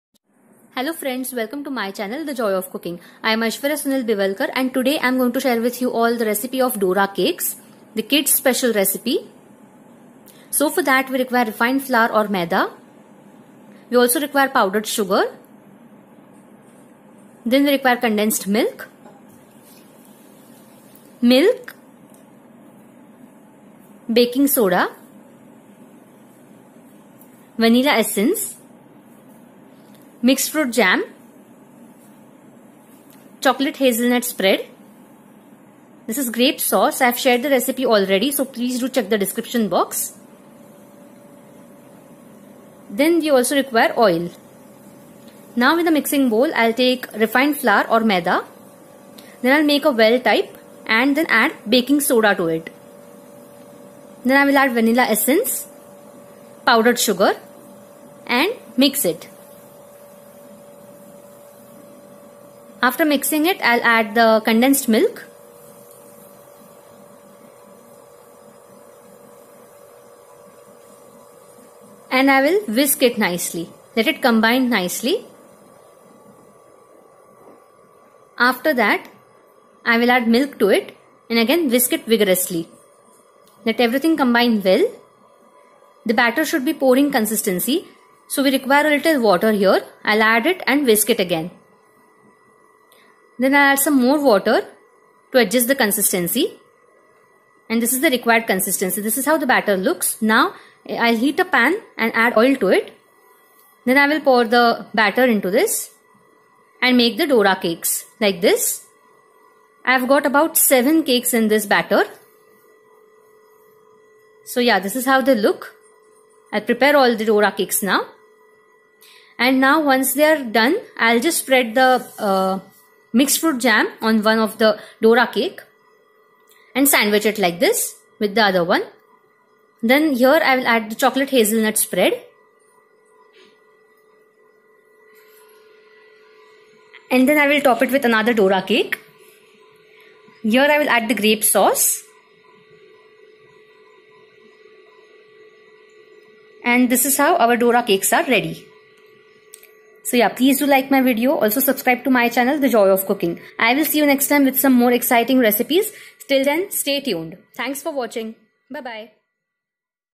Eggless Dora cakes|Doriyaki cakes|Dora pancakes|Recipe by Aishwarya Sunil Bivalkar
Hi guys!!

   Dora cakes or Doriyaki cakes

Ingredients-
1 cup Refined flour
½ cup Powdered Sugar
½ tsp Baking Soda
¼ cup Condensed milk
1/3 cup Milk
¼ tsp Vanilla Essence
Chocolate hazelnut spread
Mix fruit jam
Grape sauce

Method-
Take maida, powdered sugar, baking soda, condensed milk, milk, vanilla essence in a bowl. Whisk everything together. Add water to make the batter pouring consistency.
Heat a pan and oil to it. Then pour the batter and make small pan cakes.
Fry them for about a minute on each side or until they become golden brown in color.
Spread the mixed fruit jam, chocolate hazelnut spread and grape sauce on three  pan cakes each and top it with other pan cake.
Dora cakes are ready. Serve.

Happy Cooking!!